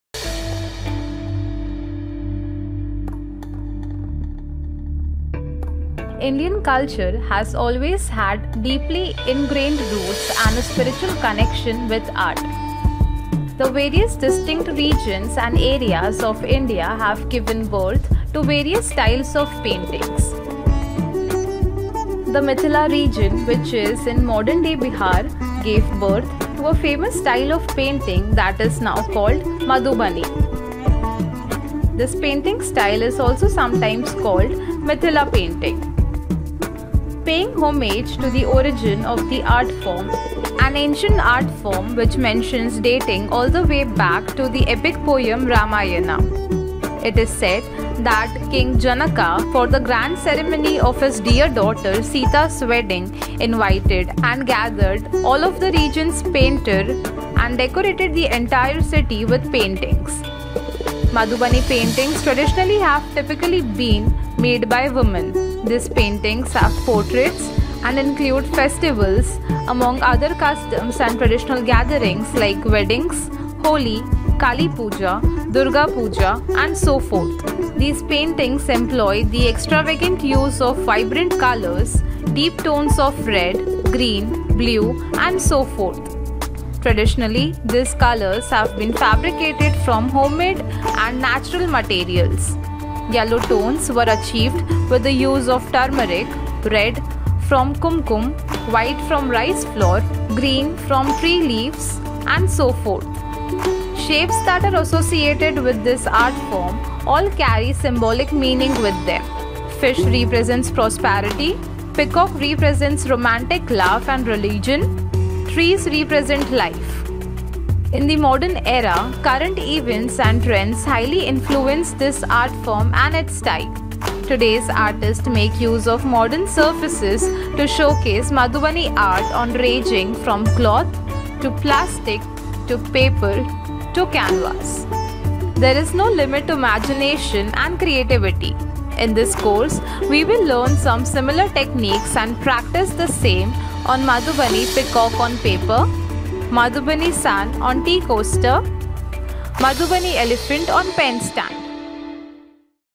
Penkraft Online Madhubani Art Course
Learn Online with Penkraft - MadhubaniPainting!

Penkraft Online Madhubani Painting course is a structured, modulebased 20-hours certified course to be completed in 25 days.

During this course, the student starts by learning the symbols used in this art form like fish, peacocks, trees, etc. The student thereafter learns how to make different types of designs, how to use colors, how to handle the brush, and to decorate different surface materials, such as paper, cartridge papers, canvas, and MDF board. This course also teaches students the correct painting techniques needed to complete these paintings and walks them through the process from scratch!

The fee for this course is INR 1499/- Material needed for the course is supplied FREE of cost and delivered to your address.